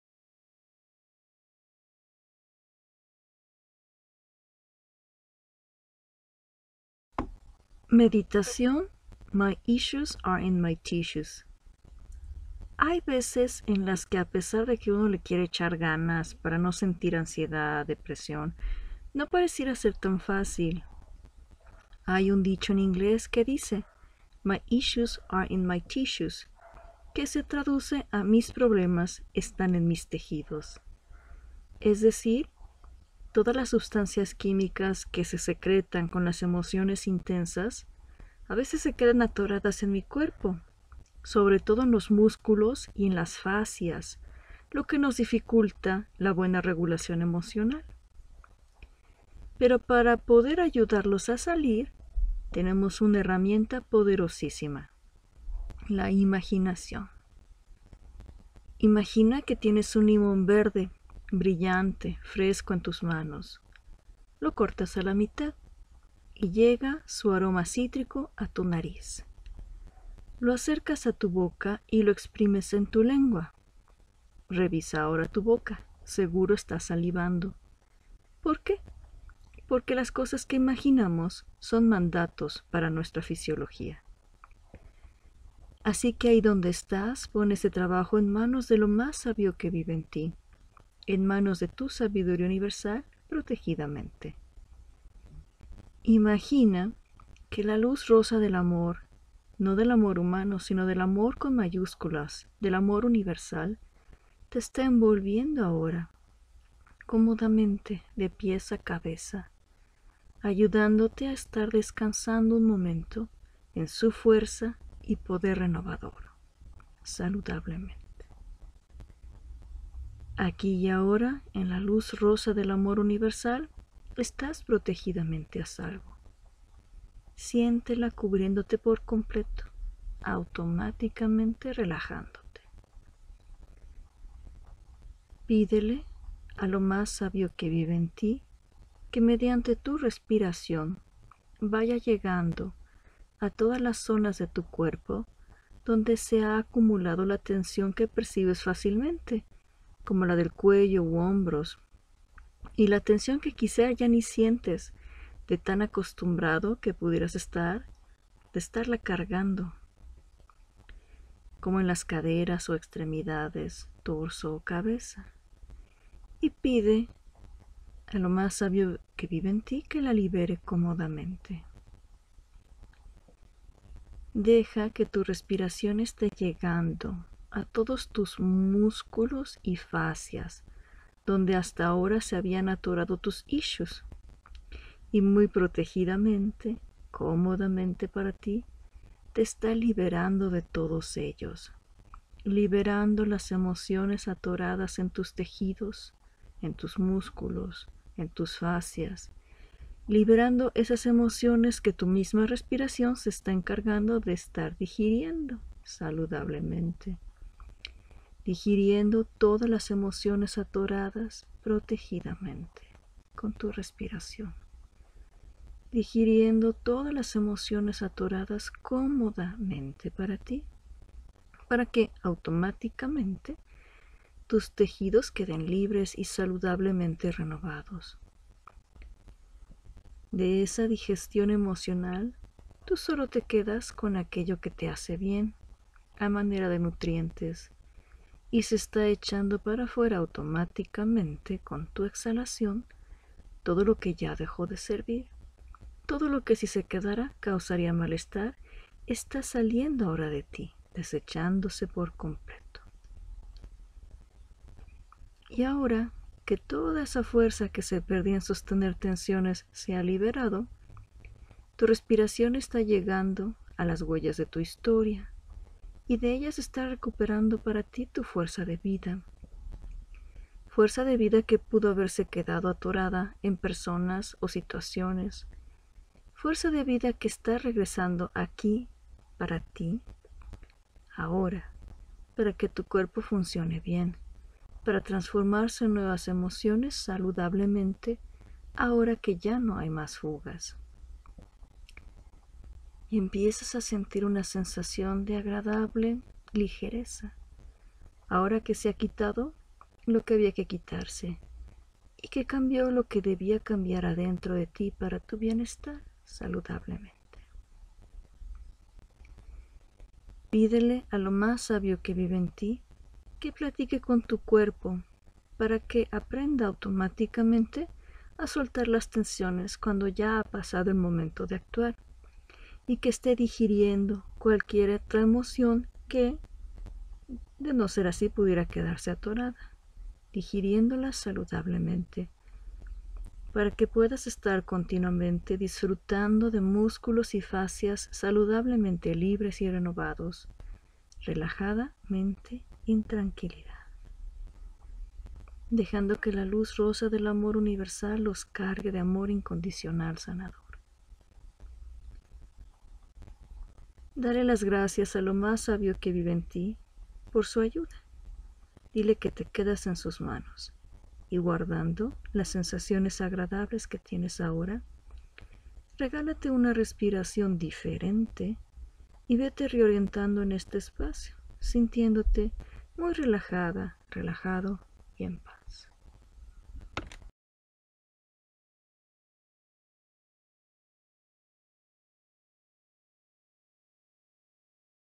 Meditación My Issues are in my Tissues
Meditación dedicada a Marcela Quezadas, amiga entrañable. Autor y voz: Flor Montero, de Psicoterapia Mindful.